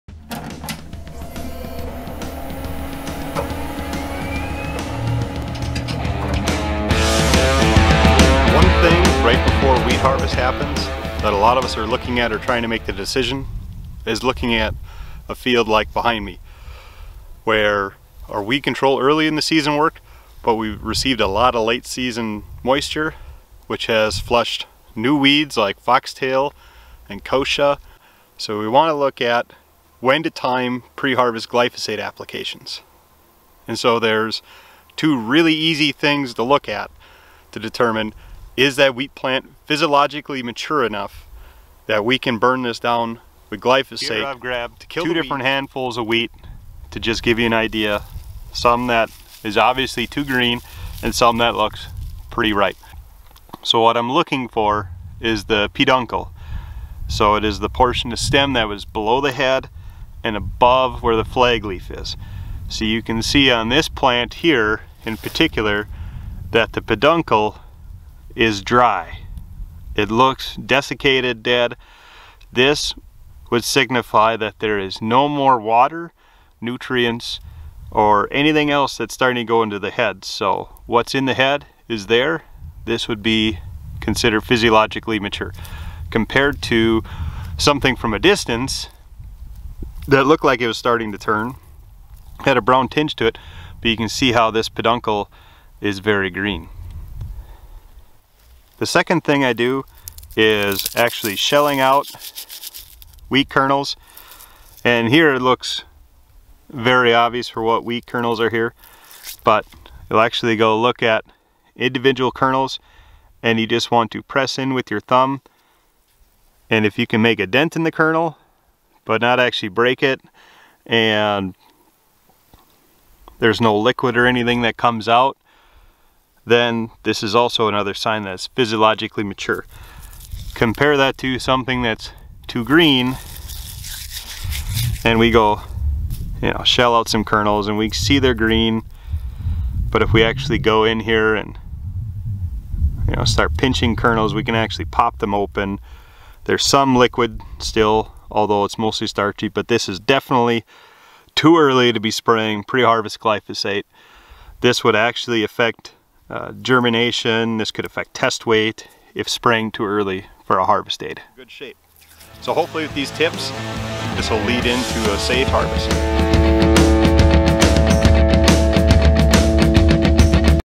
Timing Pre-Harvest Glyphosate in Wheat
How to determine when wheat has reached physiological maturity to apply glyphosate pre-harvest to wheat.